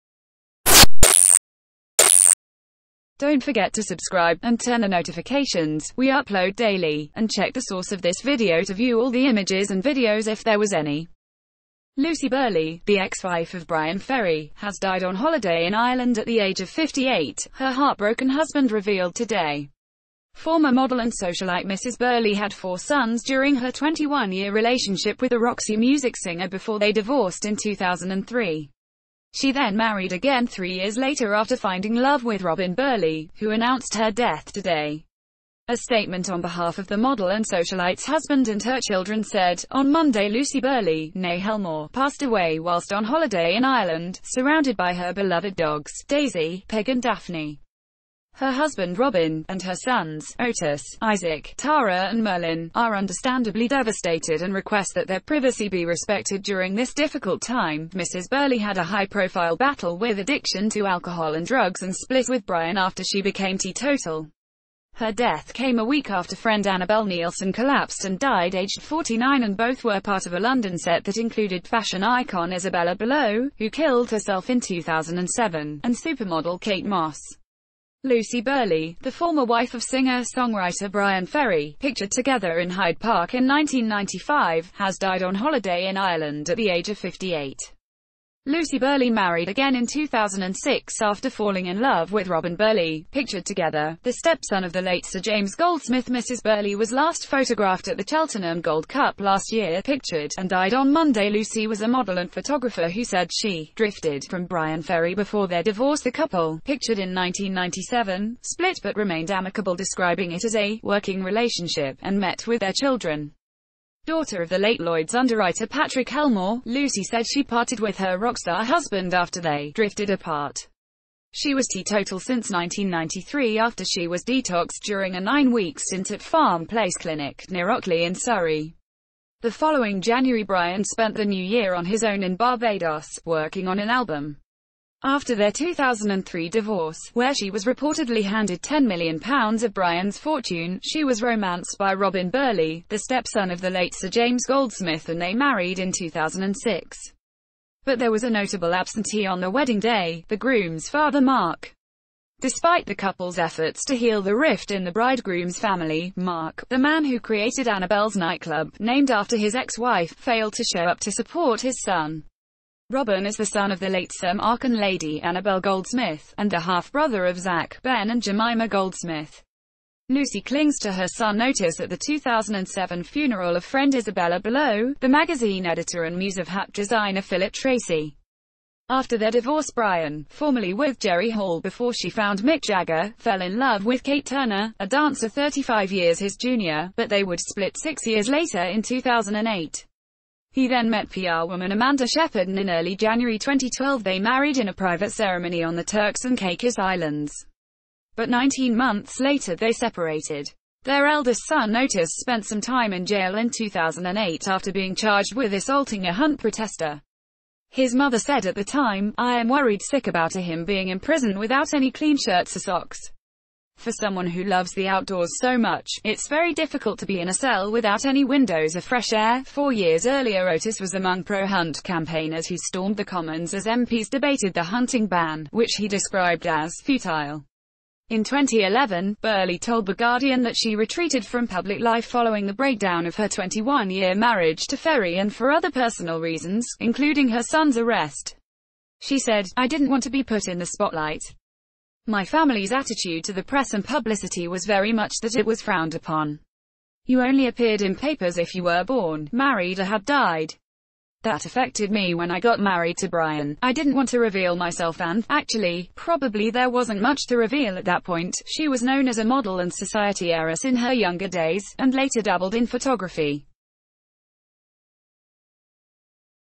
BREAKING! Bryan Ferry's ex–wife Lucy Birley dies on holiday aged 58
Source DONT' FORGET TO SUBSCIBE TO OUR CHANNEL Lucy Birley - the ex-wife of Bryan Ferry - has died on holiday in Ireland at the age of 58, her heartbroken husband revealed today.

Former model and socialite Mrs Birley had four sons during her 21-year relationship with the Roxy Music singer before they divorced in 2003.

She then married again three years later after finding love with Robin Birley, who announced her death today.

A statement on ...